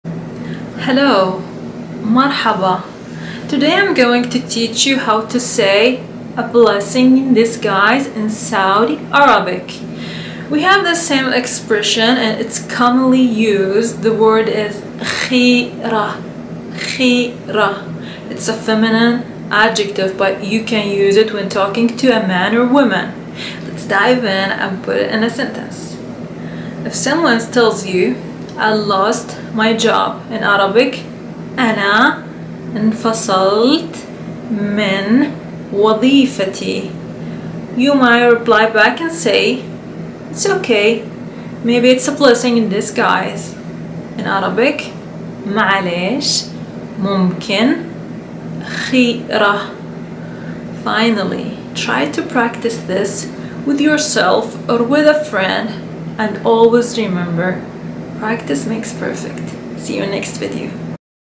Learn Saudi Arabic - "A Blessing in Disguise"
Lesson 1: Arabic Expressions - "A Blessing in Disguise"

Hi guys, in this video I want to teach you how to say a common Saudi expression, "a blessing in disguise".

اهلا،
في هذا الفيديو راح نتعلم تعبير سعودي مهم "خيرة"